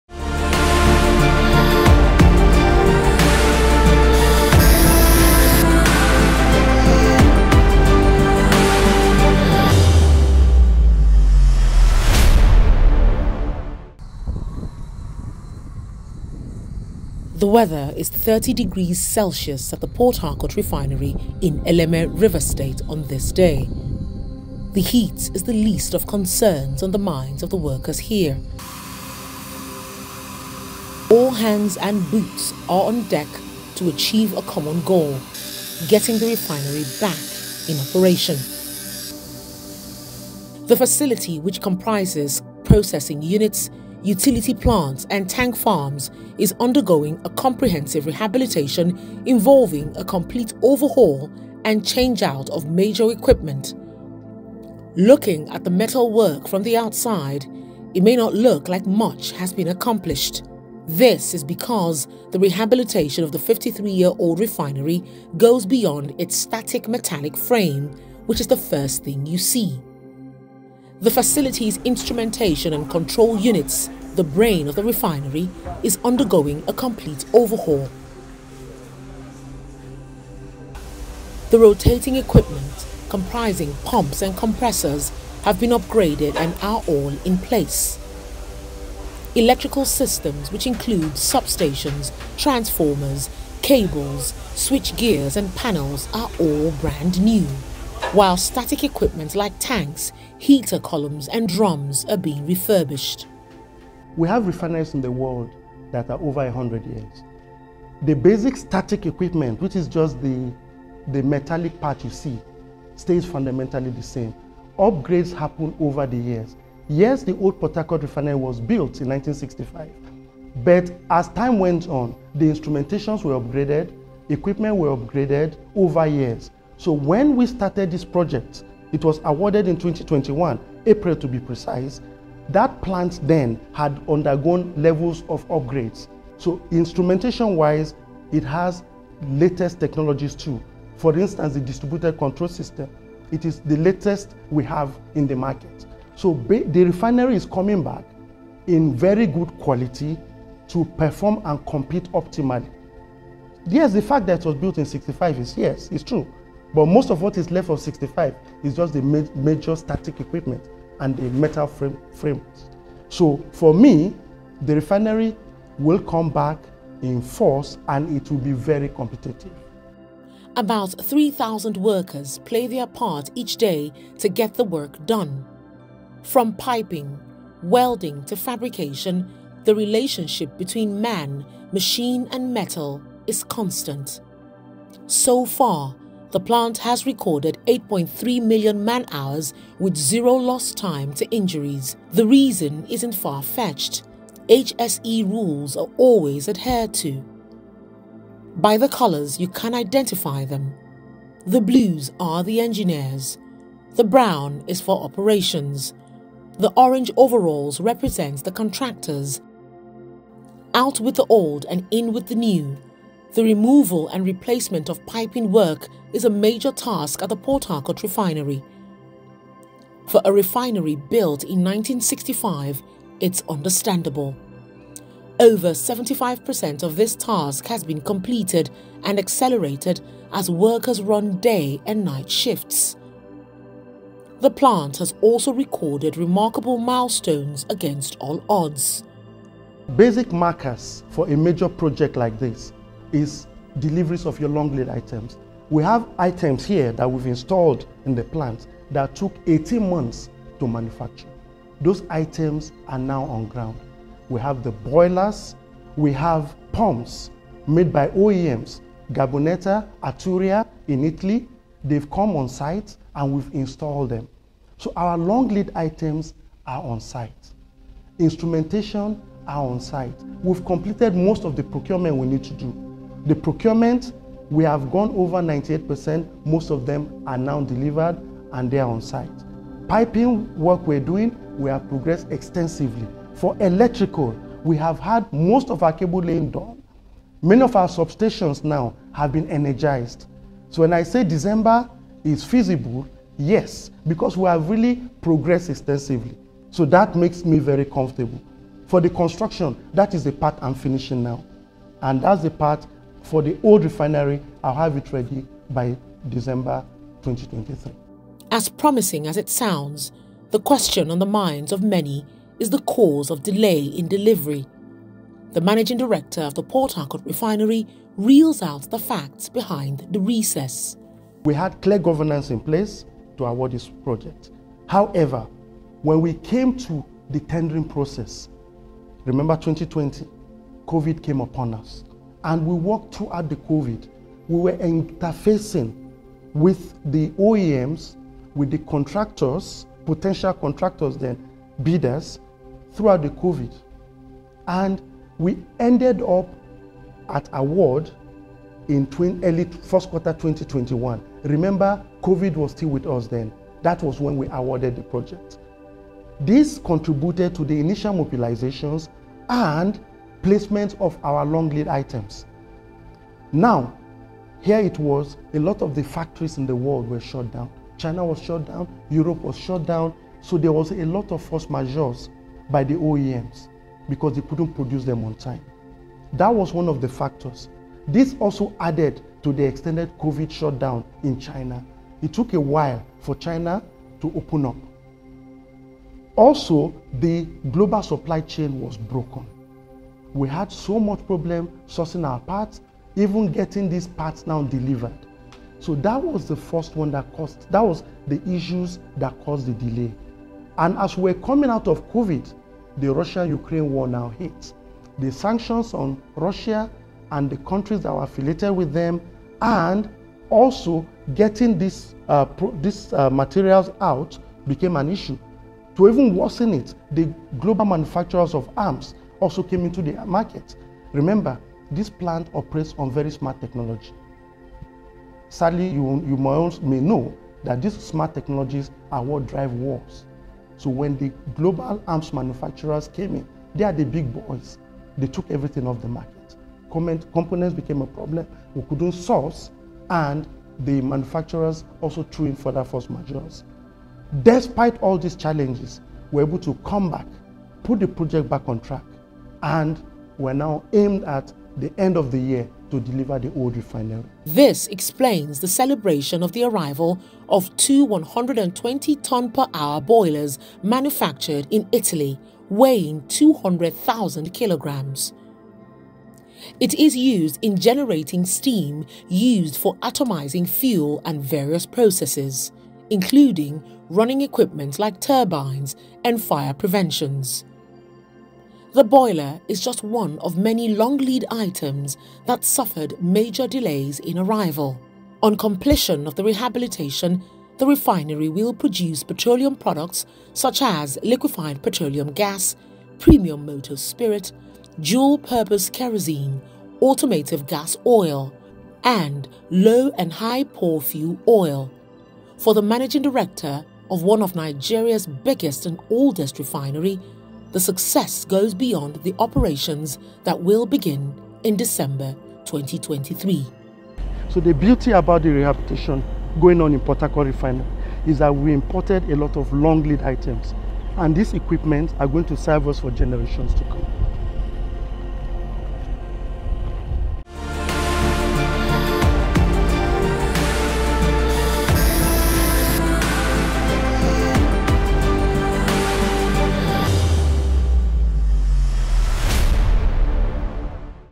PH REFINERY UPDATE DOCUMENTARY
The Port Harcourt Refinery, which comprises processing units, utility plants, and tank farms is undergoing a comprehensive rehabilitation involving a complete overhaul and change-out of major equipment.

Catch up on the latest with our series on the rehabilitation of Nigeria’s oldest refinery.